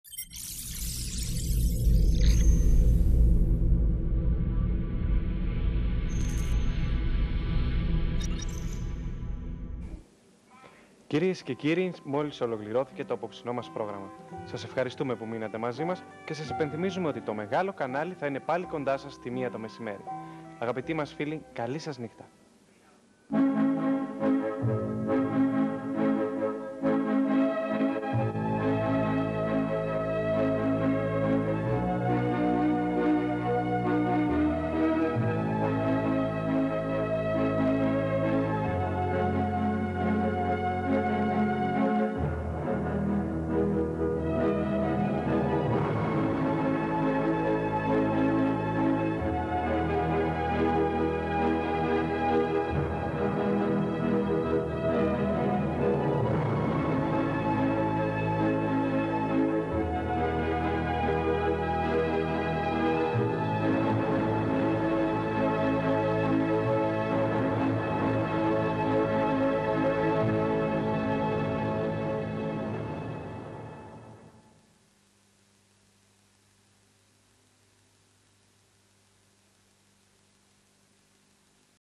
MEGA ΤΕΛΟΣ ΠΡΟΓΡΑΜΜΑΤΟΣ
Το τέλος του προγράμματος από τον τηλεπαρουσιαστή του MEGA CHANNEL.
Μεταδόθηκε 07 Ιανουαρίου 1990.